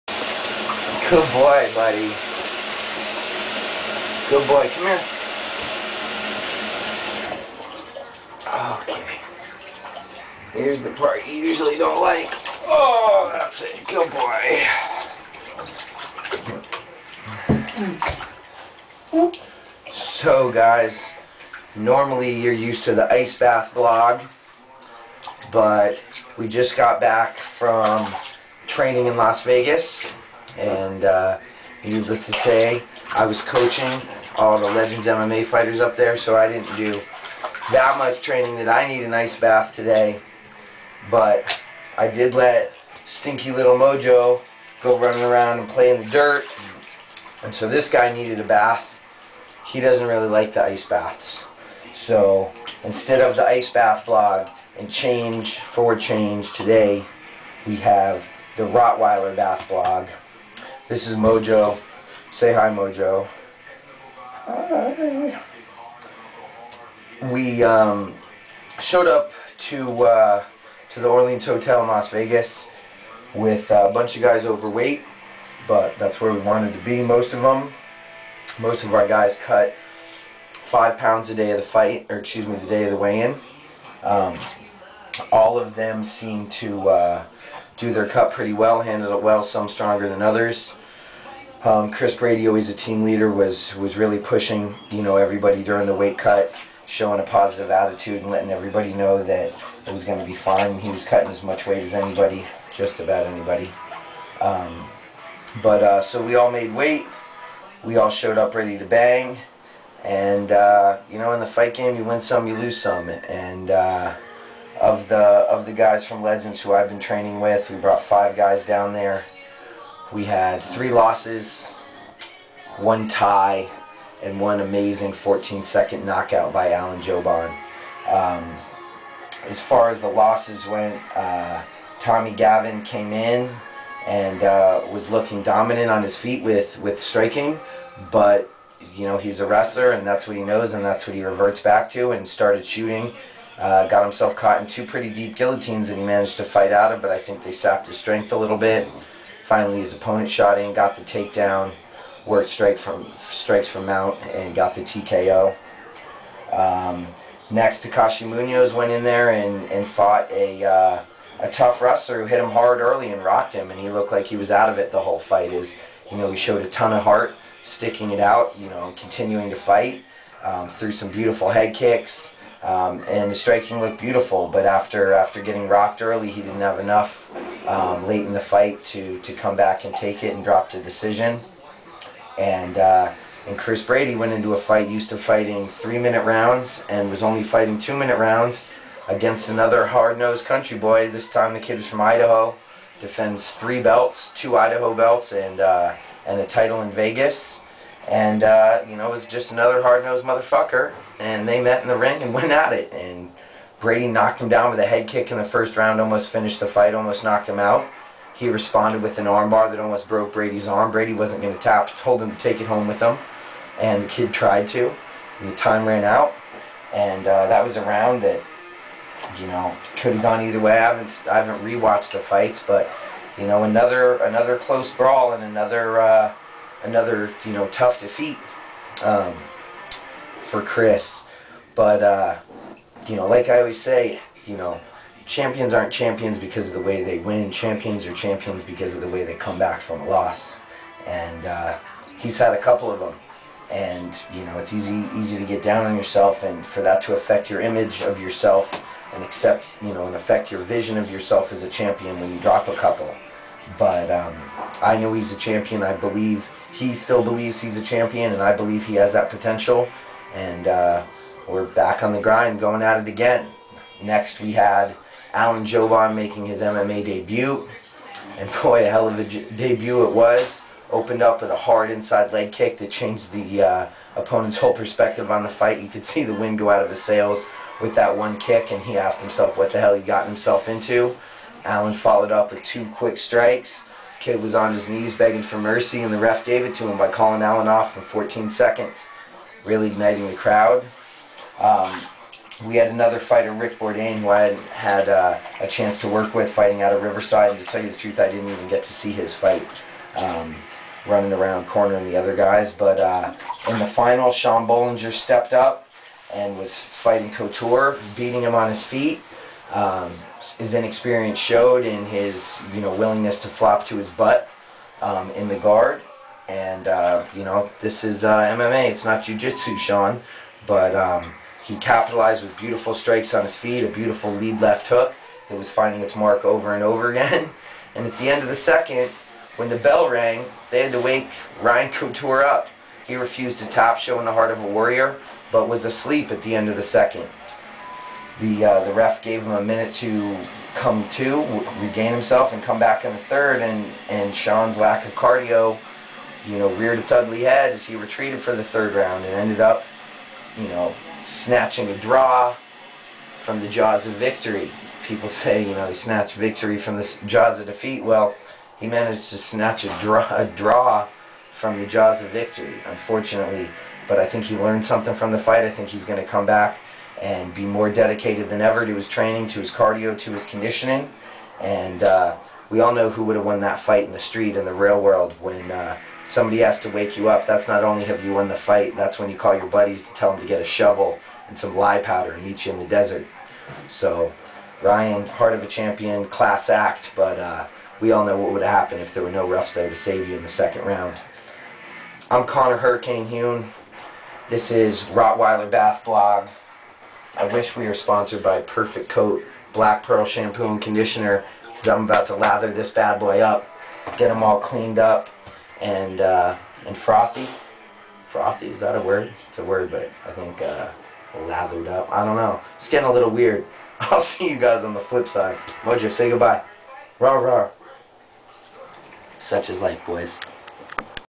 RottweilerBathBlog.MPG
Following the Tuff-Nuff Fights in Las Vegas on March 26th I forgo the usual Ice Bath Blog in favor of a bath with my dirty rottweiler Mojo.  I reflect on the fights while relaxing and lathering up my furry friend.  This is a nice relaxing change of pace for me from the usual pit of pain that my tub becomes when filled with 40 lbs of ice.  Enjoy folks, and perhaps pick up some tips on canine care and a nice way to clean you dog.